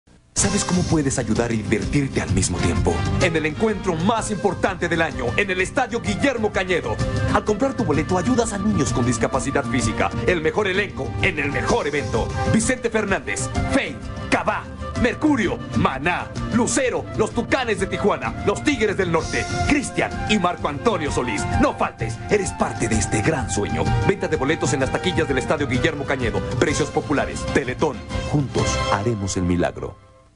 Comerciales mexicanos: TELETON 1997
Concierto de apoyo al Teleton en el estadio Guillermo Cañedo